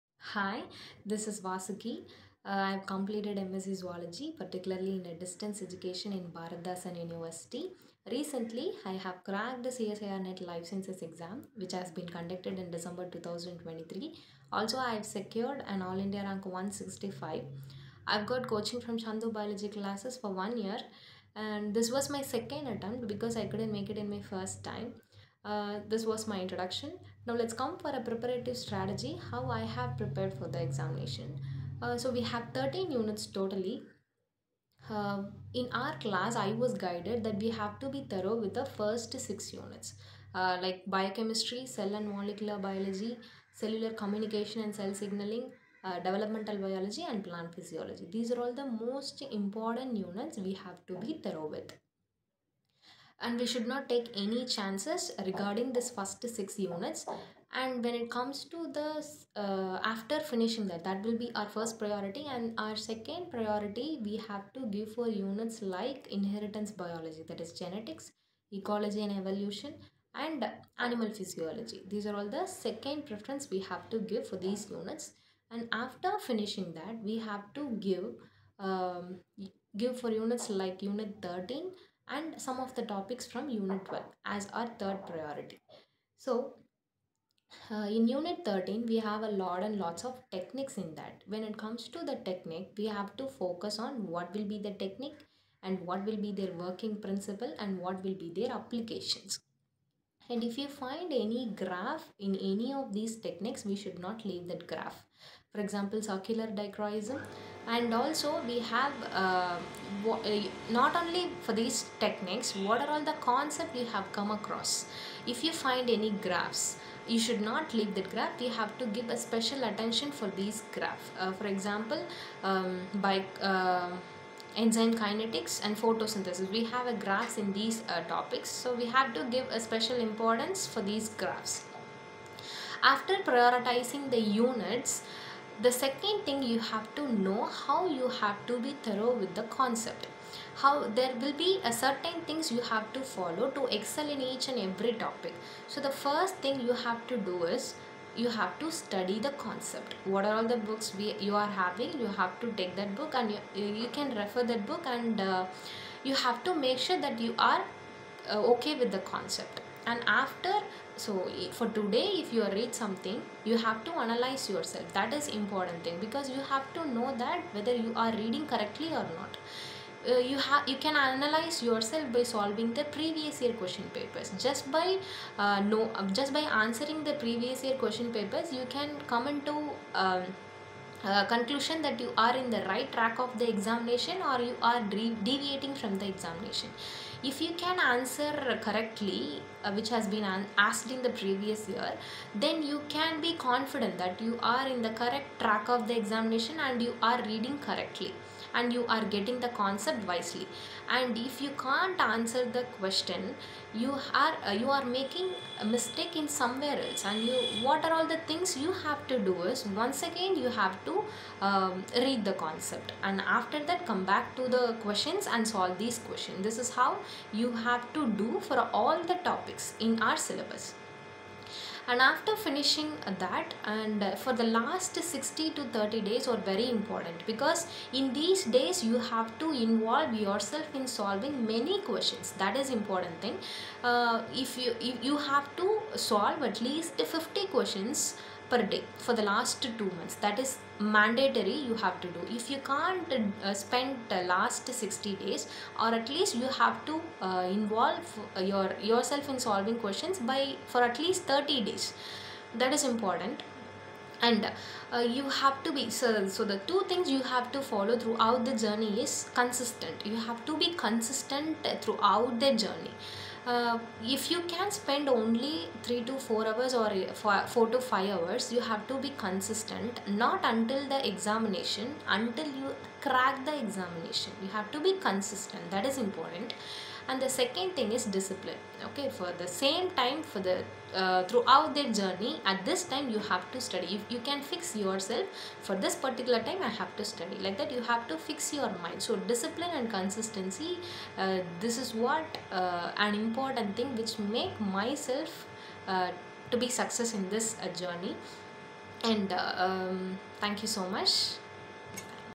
How Vasuki.R Achieved Rank 165 in CSIR NET Life Sciences With Our Expertise
Elated to announce that our student Vasuki.R has secured an incredible All India Rank (AIR) 165 in CSIR NET Life Sciences!  This video features Vasuki.R sharing her journey to success and the valuable insights she gained along the way.

Are you aiming to crack CSIR NET Life Sciences?  This video is packed with practical tips and strategies that helped Vasuki.R achieve her dream rank. You'll learn about:

Her effective study plan and approach
The resources and tools she found most useful
How Coaching Dream Achievers supported her success
Tips for staying motivated and overcoming challenges
If you're serious about cracking CSIR NET Life Sciences, don't miss this inspiring conversation with Vasuki.R!  Get ready to be motivated and gain valuable insights for your own exam preparation.